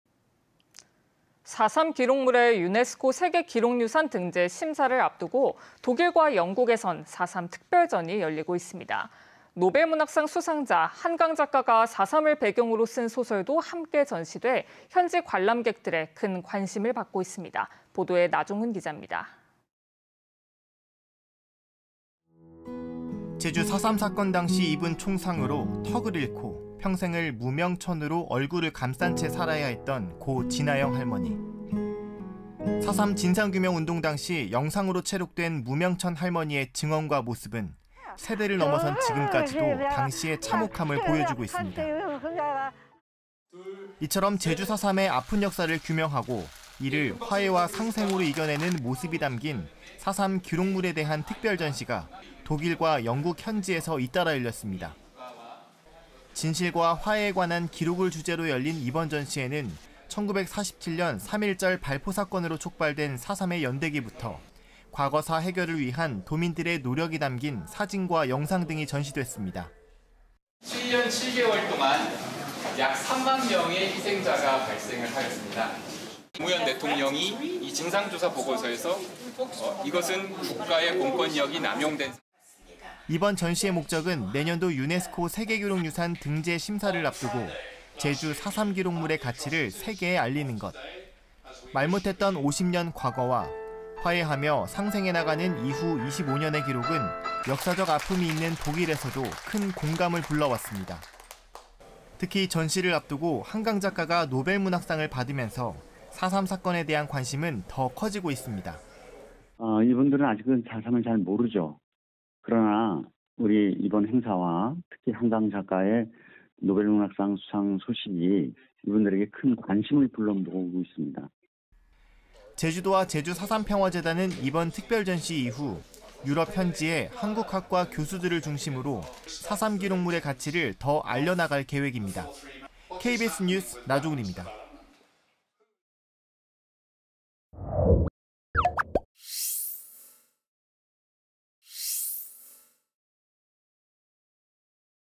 유럽에 전하는 ‘제주 4·3’…“과거사 해결의 모범” / KBS 2024.10.18.
독일 베를린과  영국 런던 현지에서는 제주 4·3 기록물의 가치를 알리기 위한 특별 전시가 열리고 있습니다.
내년도 유네스코 세계기록유산 등재 심사를 앞두고  전 세계인들의 공감대를 이끌어내고 있습니다.